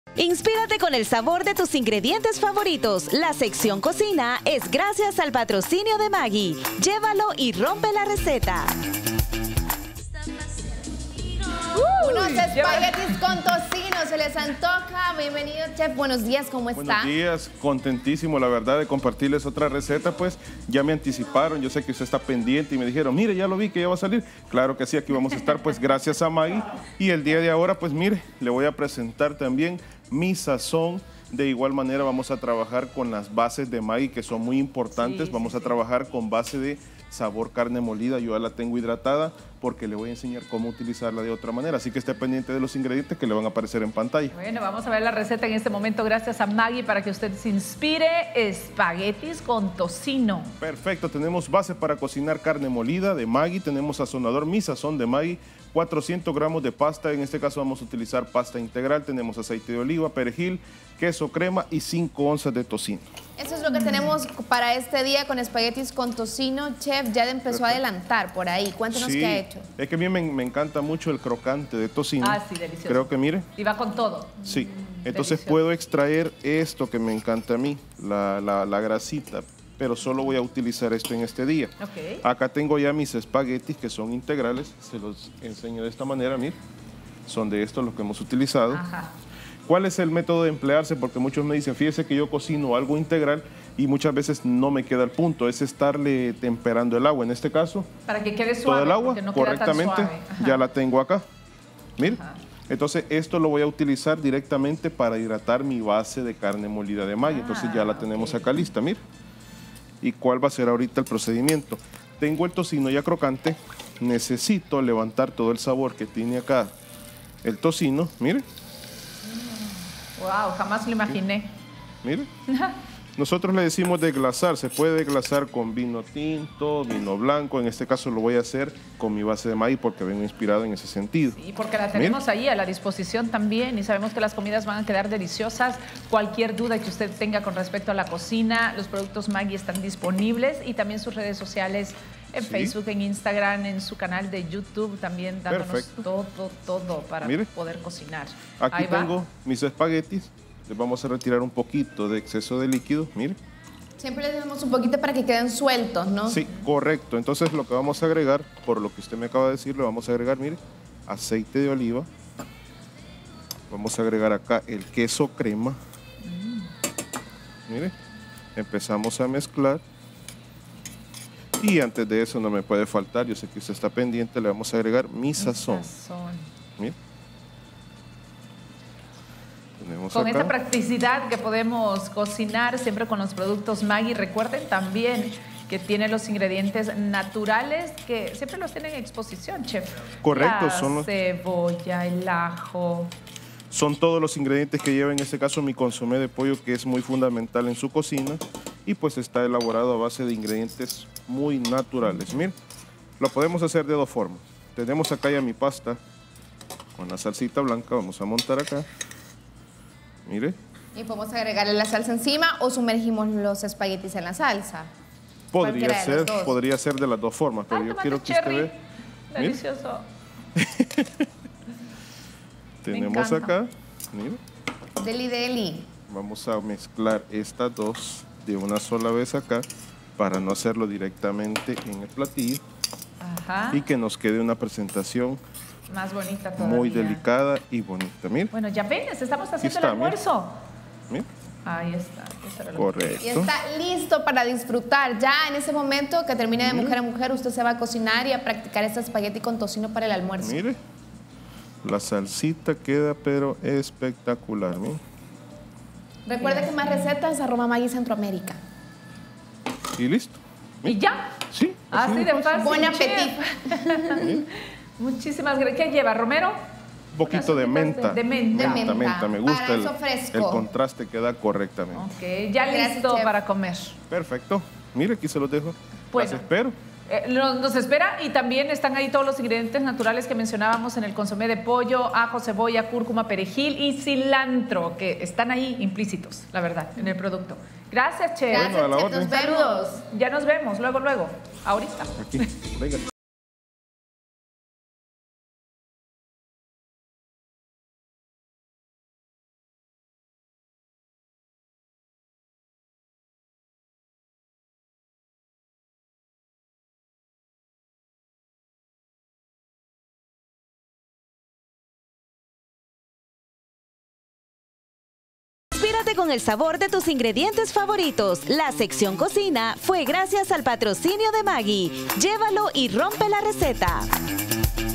DeMujerAMujer | Espaguetis con tocino
Hoy en la cocina con MAGGI preparamos unos deliciosos  “espaguetis con tocino”. Te compartimos la receta para que tú los puedas preparar 😋